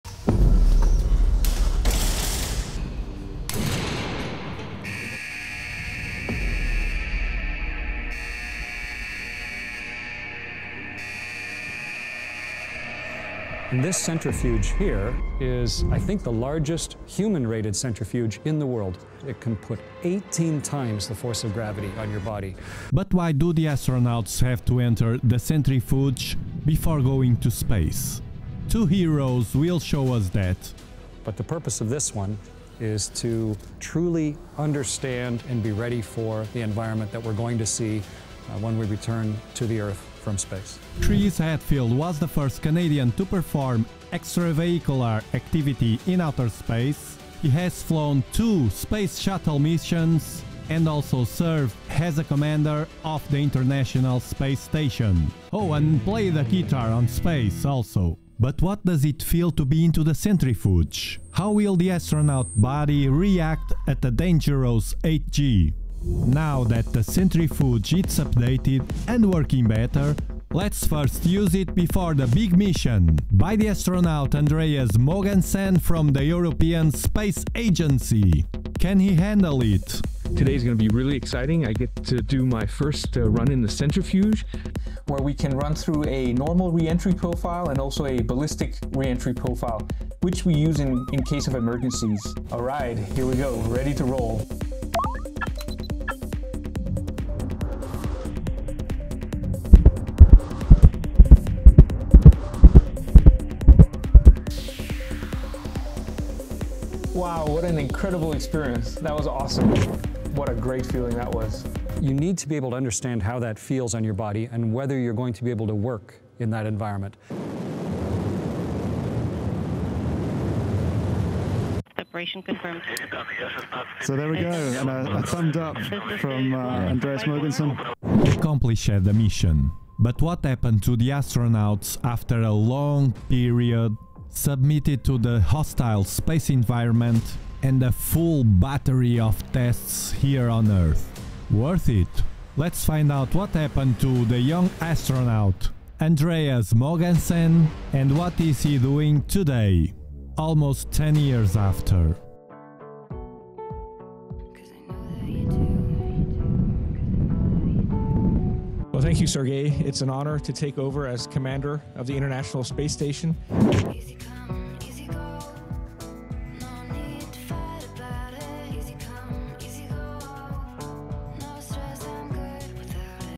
Russian G-MONSTER Centrifuging Astronauts at 8-G!
The Centrifuge is an essential component of astronaut training programs, designed to prepare astronauts for the physiological challenges they will face during spaceflight.
These astronauts experimented 8-G on the world's largest Russian Centrifuge.